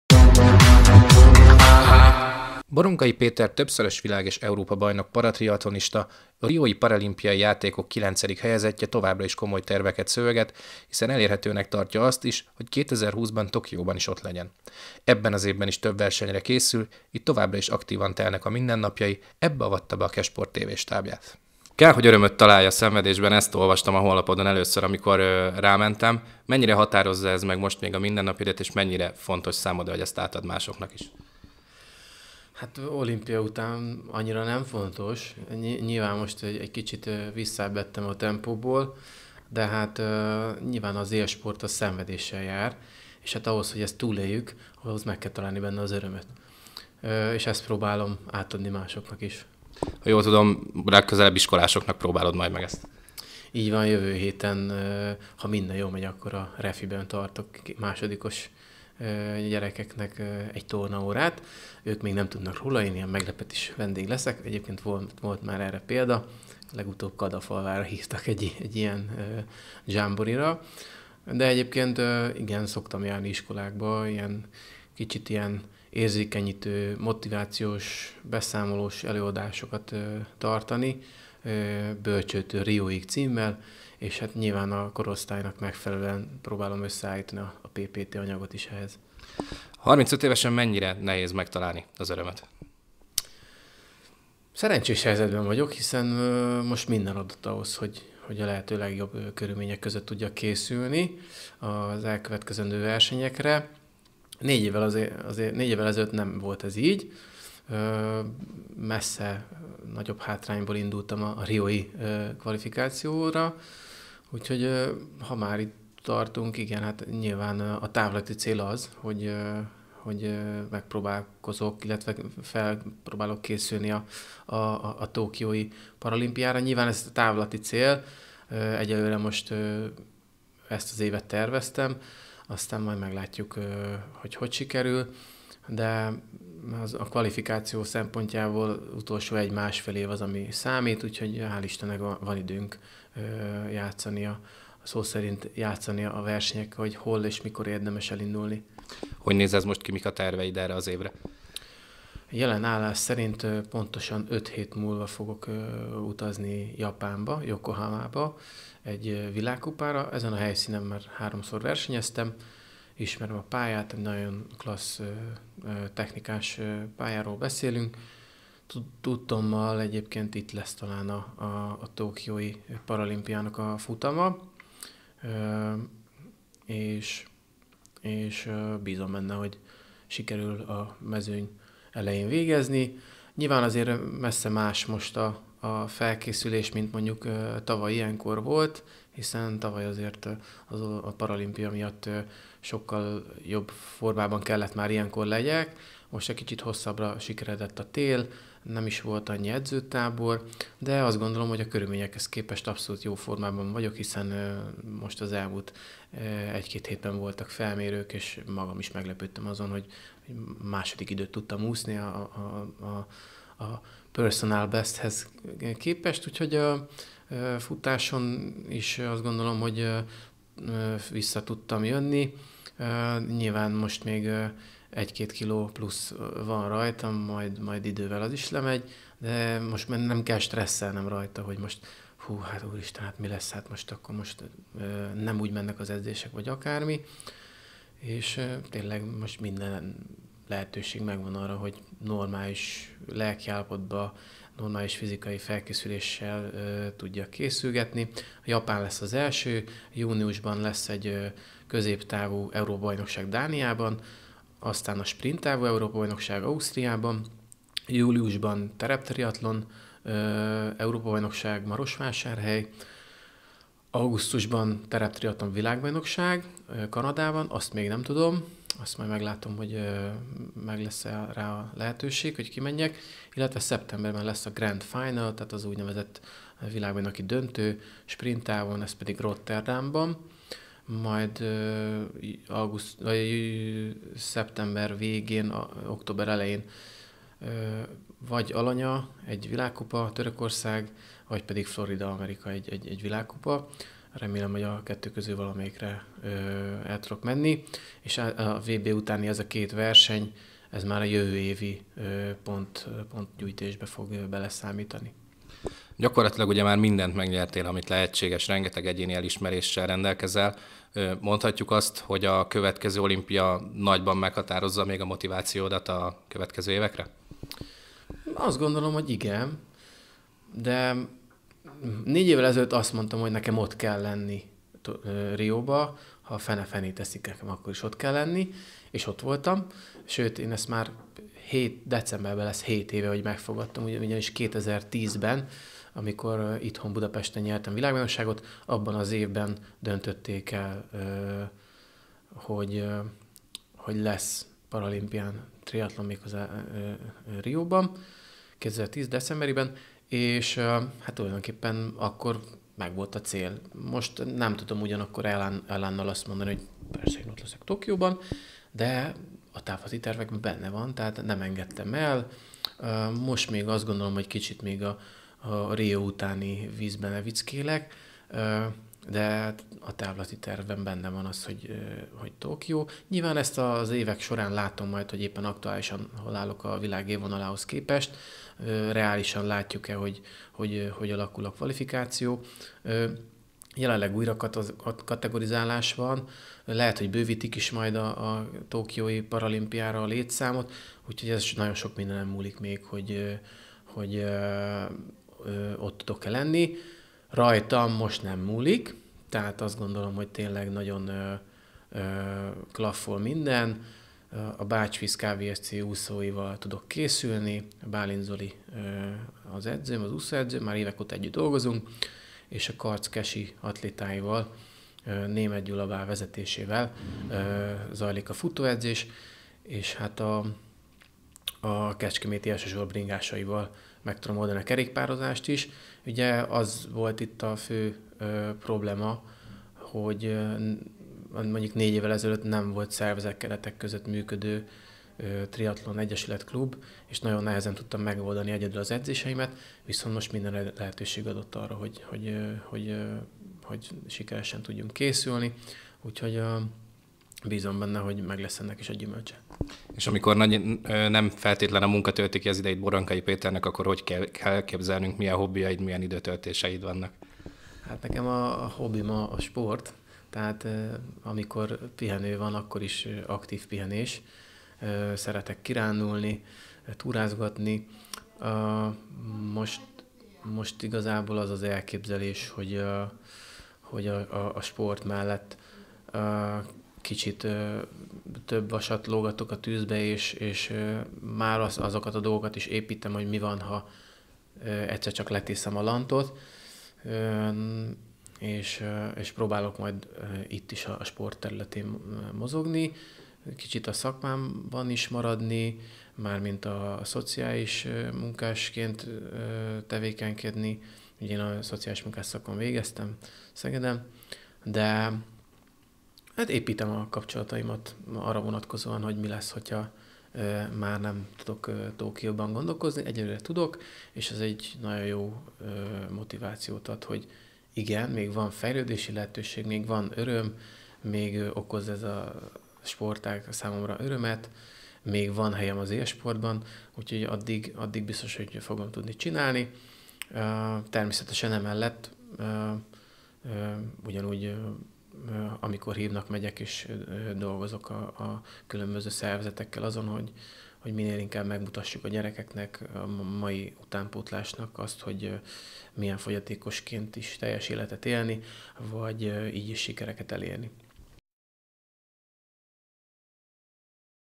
"Kell, hogy örömöt találj a szenvedésben" - Boronkay Péternél jártunk
Boronkay Péter többszörös világ- és Európa-bajnok paratriatlonista, a Mercedes-Benz Gyàr KMSE versenyzője, aki 2016-ban a riói paralimpián is ott lehetett nagy álmát valóra váltva. Most már 2020-ra készül, ám idén is több versenye lesz. A Kesport TV bele leshetett egy napjába.